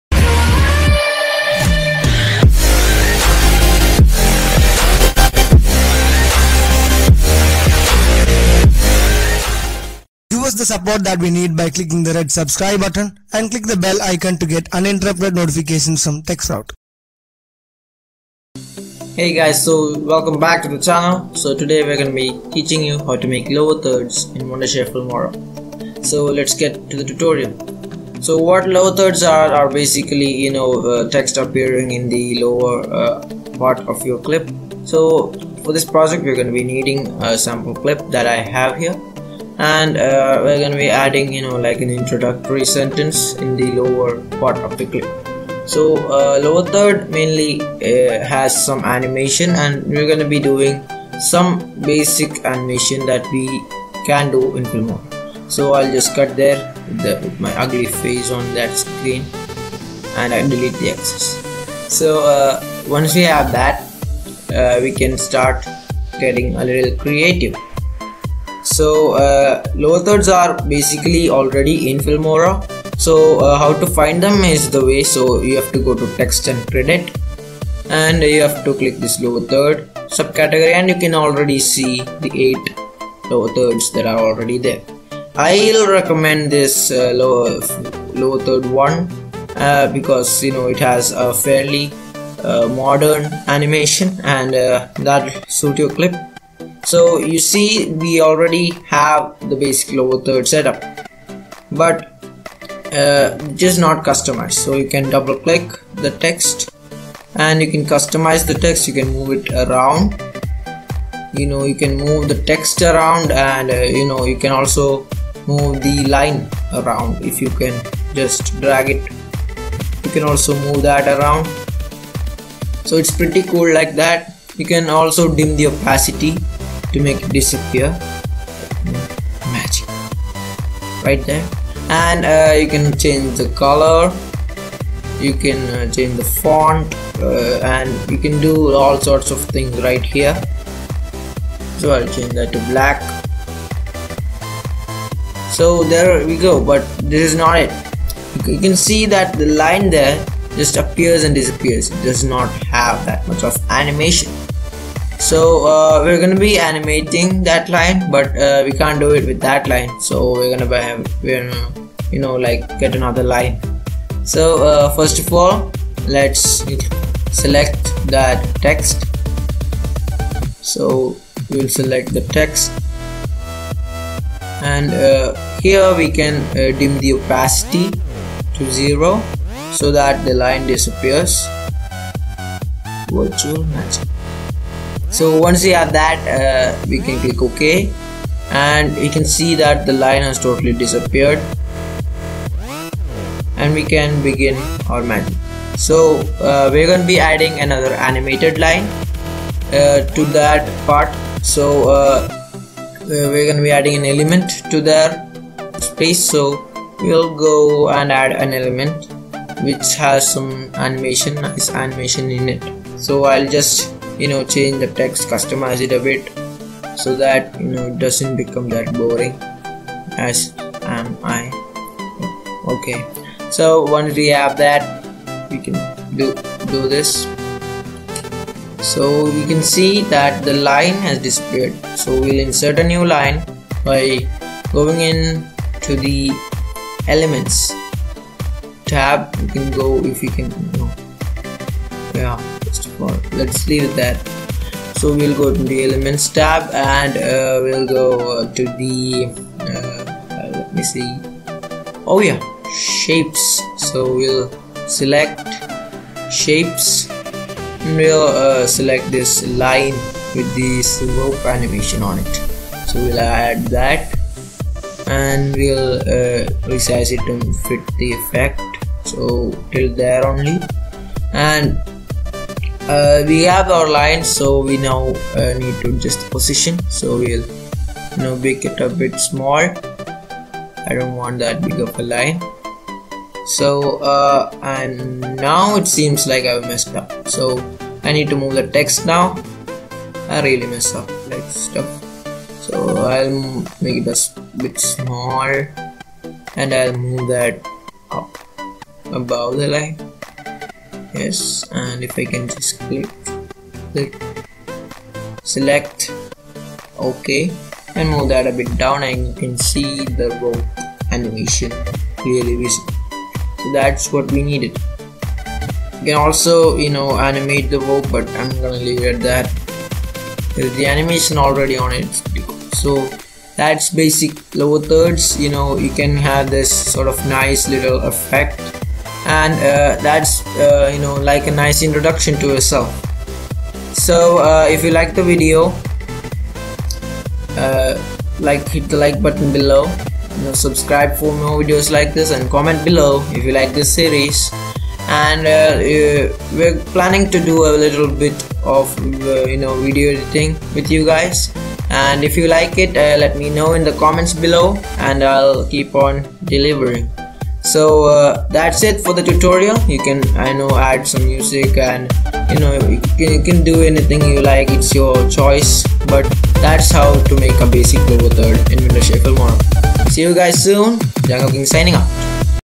How To Make Lower Thirds In Filmora | Video Editing Tutorial | #3
Hey Guys,
Welcome back and today I'm gonna show you guys how we can make cool looking Lower Thirds In Filmora. If you guys didn't know Filmora is a basic video editing software from Wondershare.
This is the 3rd episode In this series and I hope you guys are loving It.